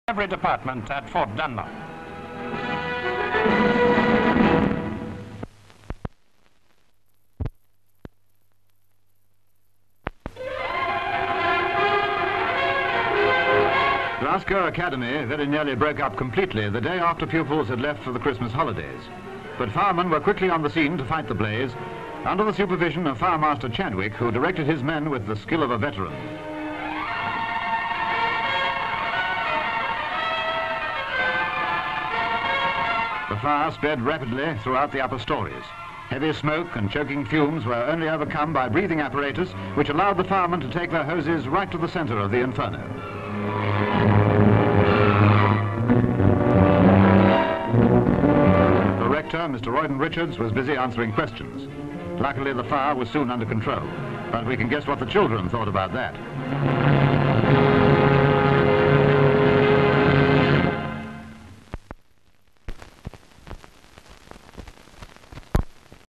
GLASGOW ACADEMY FIRE
GV of the fire. GV of the building. Scenes of firemen. Hoses. Mr Roydon Richards talking to the press. Firemaster Chadwick directing operations. Children looking on. CU of fireman with breathing apparatus.